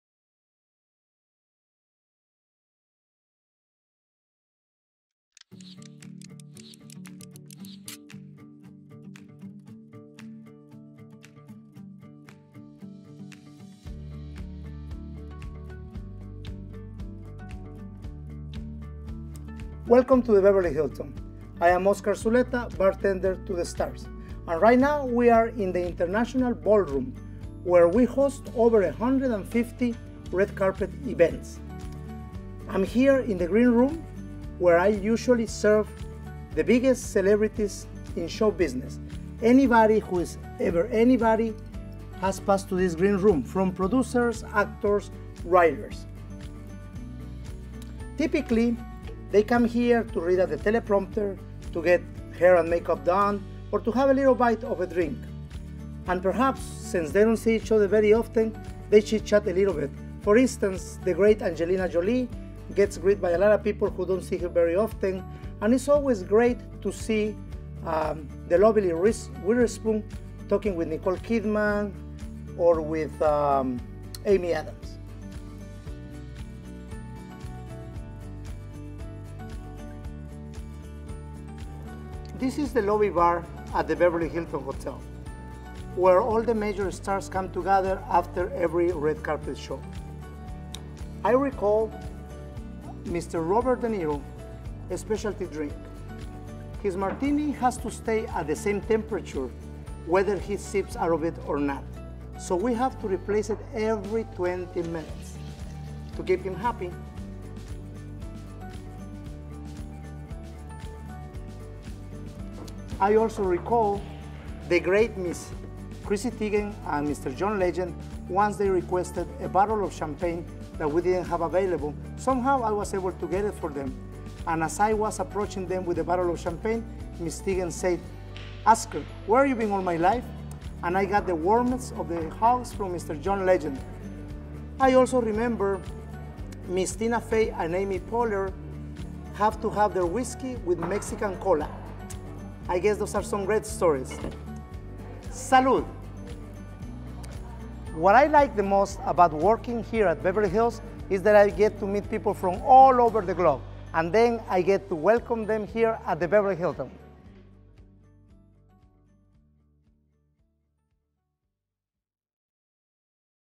A 360° Behind the Scenes Visit at The Beverly Hilton
Take a 360° look into the lives of the individuals who are redefining luxury and service in Beverly Hills.

Opened in 1955 by Conrad Hilton, the Forbes Four-Star hotel combines the excitement of Hollywood with the prestige of Beverly Hills. Nestled at the crossroads of the iconic Wilshire and Santa Monica Boulevards, the hotel’s 569 guest rooms evoke the best of relaxed California living, including 101 suites with panoramic city views and inviting indoor-outdoor living spaces, as well as nine private luxury suites within The Penthouse Collection.

Oscar Zuleta, The Beverly Hilton bartender-to-the-stars, shares some stories and gives us a behind the scenes look into the green room used for the Golden Globes Awards.